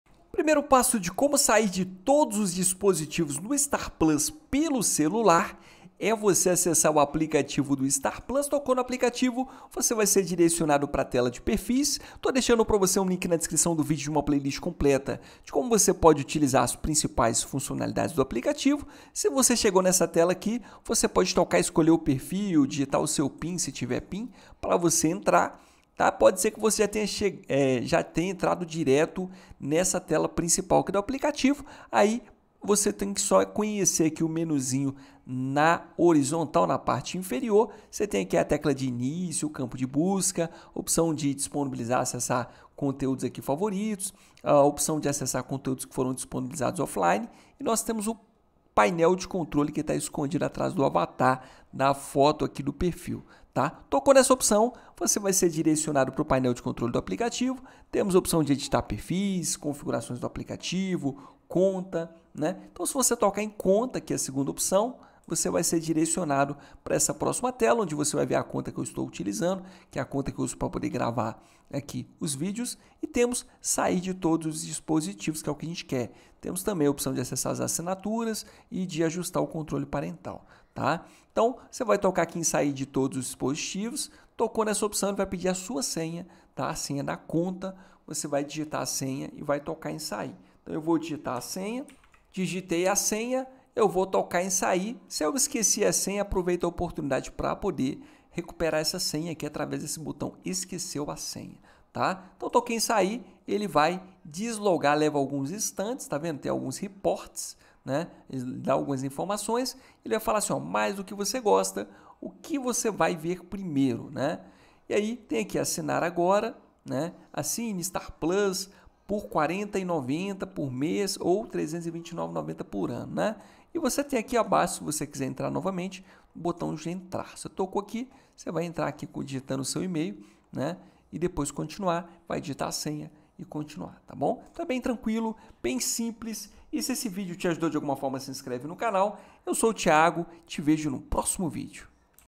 COMO SAIR DE TODOS OS DISPOSITIVOS NO STAR PLUS PELO CELULAR
⏩ No vídeo de hoje ensino você COMO SAIR DE TODOS OS DISPOSITIVOS NO STAR PLUS PELO CELULAR.

Neste vídeo, você vai aprender como sair de todos os dispositivos no Star Plus pelo celular. Isso é útil se você quiser proteger sua conta ou se você perdeu ou trocou de aparelho. Para fazer isso, você precisa seguir alguns passos simples que vamos mostrar agora.